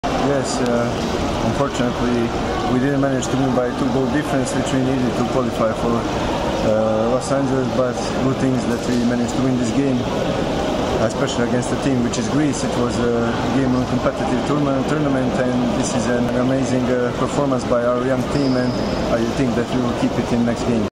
Petar Tesanovic post-match interview - Montenegro 8 - 7 Greece.
After a great game between the sticks, Petar Tesanovic tells he was impressed by his teams us win over Greece 🔥